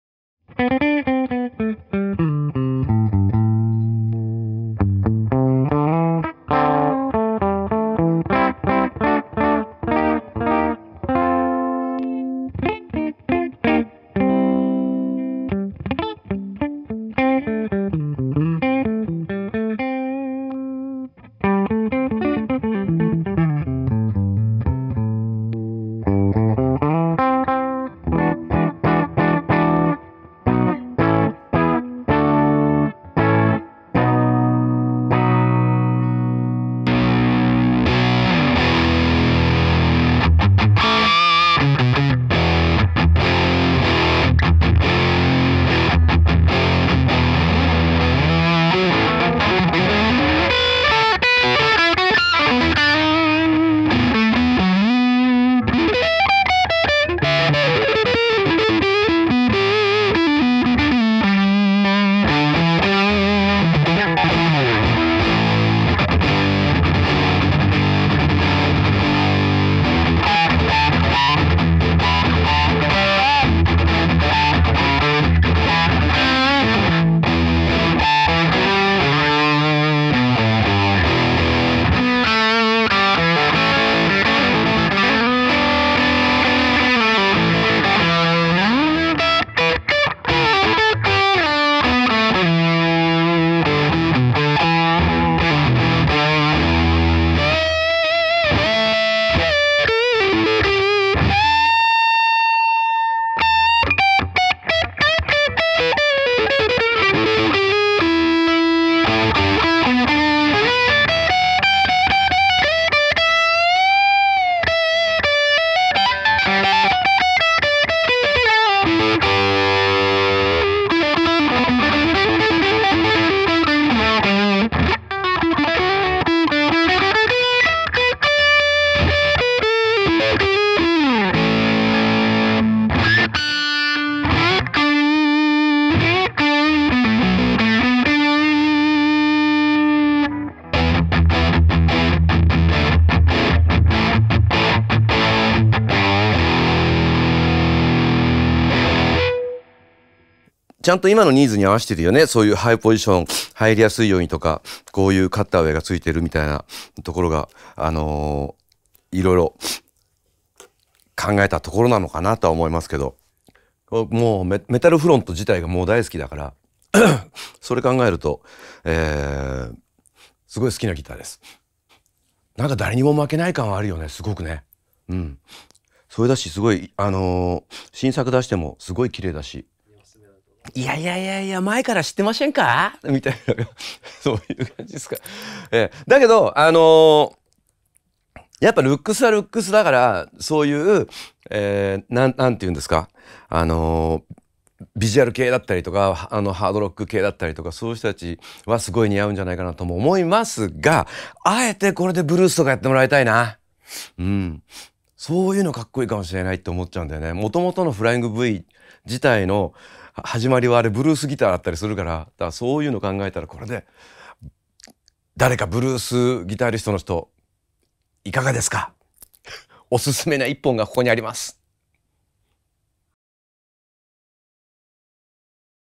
野村義男が弾く！ZEMAITIS / MFGV22 BK
◎ギター・マガジン2023年1月号（12月13日発売）連動動画
ご購入はこちらから↓↓↓

今、国産ギターがアツい。日本の技術が光る作りの良さはもちろん、各ブランドのこだわりが詰め込まれた使用木材やパーツ、そしてオリジナリティ溢れるボディ・シェイプや独自のコントロールなどを採用し、“ギタリストの好み”が細分化された現代において、初心者からベテランまで様々なプレイヤーをカバーする懐の広いギターがたくさん生まれている。まさに国産ギター群雄割拠の状態なのだ。今回はギター・コレクターとしても知られる野村義男に登場願い、注目のブランドのギター10本の実力を確かめてもらった。ぜひギター選びの参考にしてほしい。

■このギターの詳しいスペックや野村義男のコメント全文はこちら↓↓↓

■試奏機材
・Amplifier：Fender / '68 Custom Deluxe Reverb
・Effect Pedal：F-Pedals / EDSTORTION（野村義男私物）

※本動画の著作権は原則として株式会社リットーミュージックが保有し、許可なく複製、転用、改変、画像キャプチャ、販売などの二次利用を行なうことを禁じます。